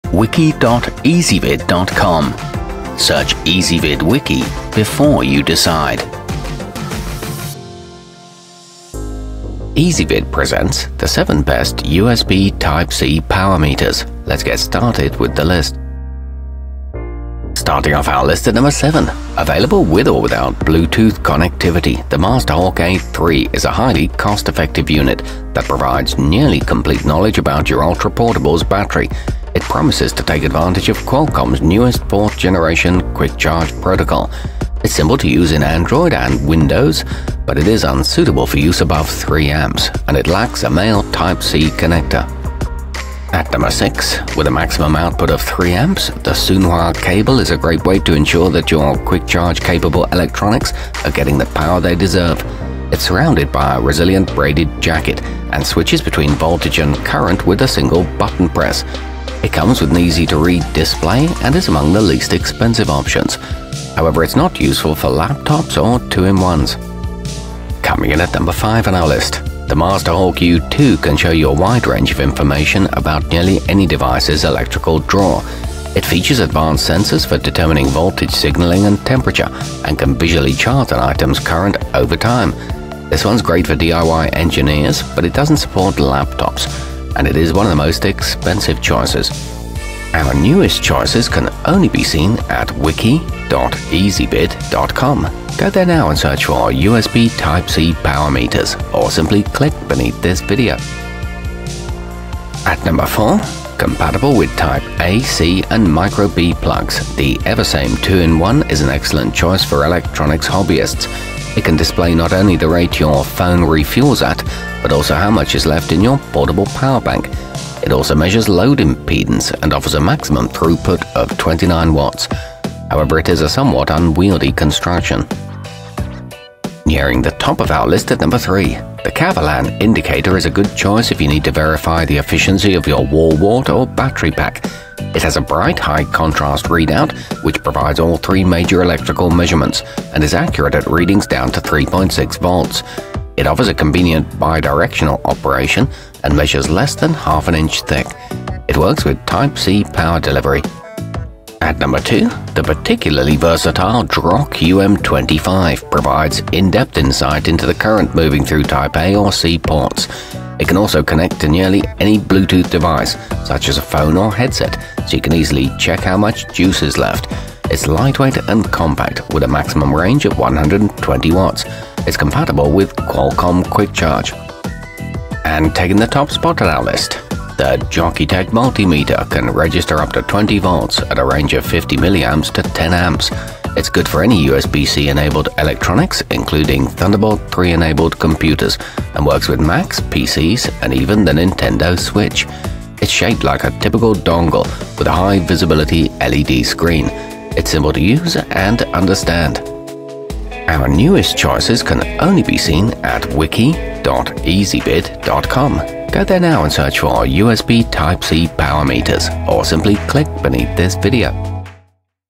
7 Best USB Type-C Power Meters 2019
Our complete review, including our selection for the year's best usb type-c power meter, is exclusively available on Ezvid Wiki.

Usb type-c power meters included in this wiki include the drok um25, kavalan indicator, jokitech multimeter, masterhawk u2, eversame 2 in 1, masterhawk a3, and soonhua cable.

Usb type-c power meters are also commonly known as usb type-c testers.